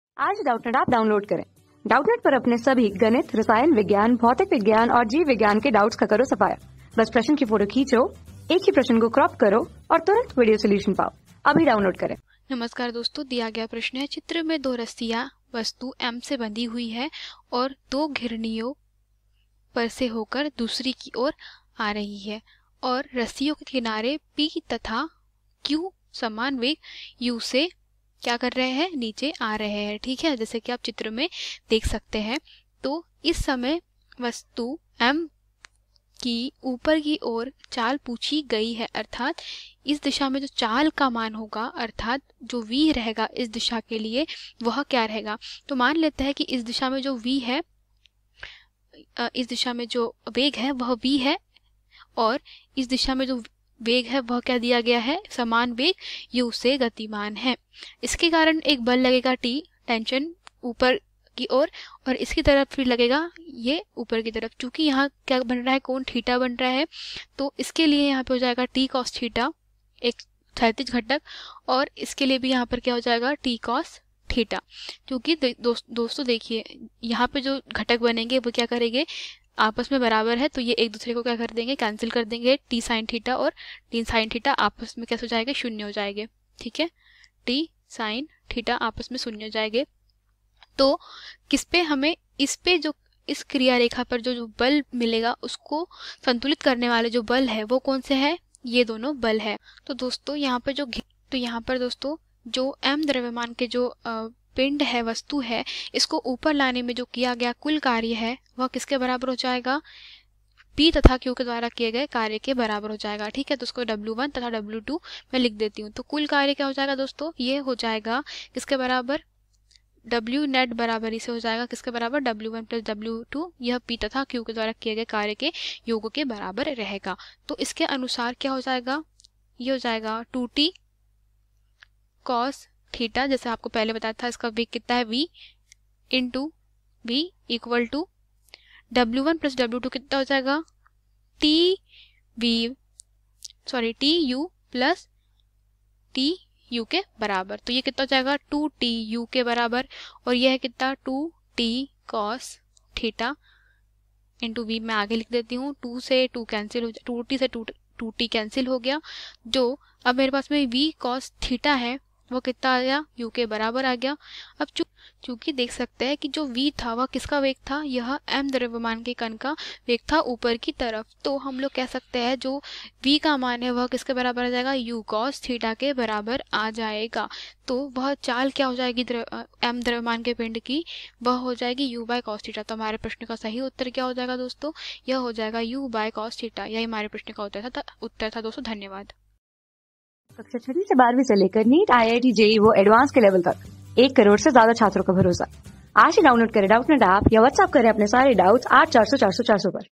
चित्र में दो रस्सियाँ वस्तु M से बँधी है ओर दो घिरनियों पर से होकर दूसरी ओर आ रही है रस्सियों के...
चित्र में दो रस्सियाँ वस्तु M से बँधी है ओर दो घिरनियों पर से होकर दूसरी ओर आ रही है रस्सियों के किनारे P तथा Q समान वेग u से नीचे आ रहे है । इस समय वस्तु M की ऊपर की ओर चाल होगी ?
Class: 12
Subject: PHYSICS
Chapter: समतल में गति
Board:Bihar Board

👉 Have Any Query? Ask Us.